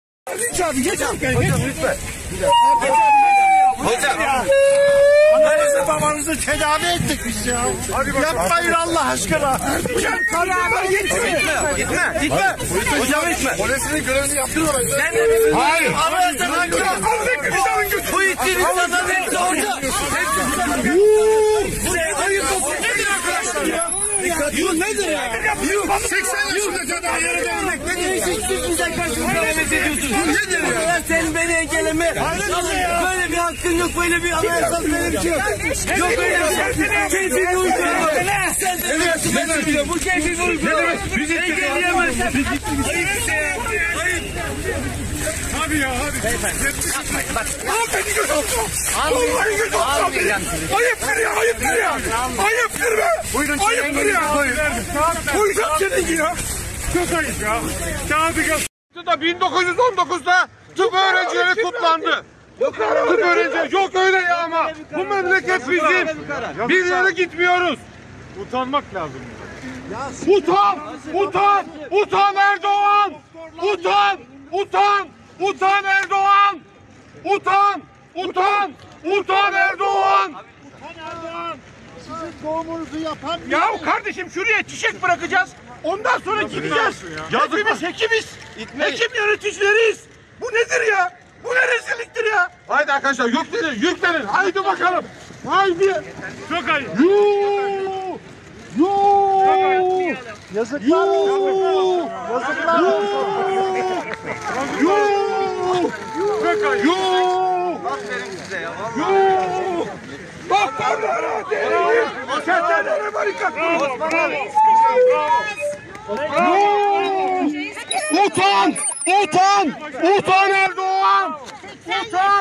Taksim'de hekimlere polis müdahalesi
14 Mart Tıp Bayramı nedeniyle Taksim'de Atatürk Atatürk Anıtı'na çelenk bırakmak isteyen hekimlere polis müdahale etti.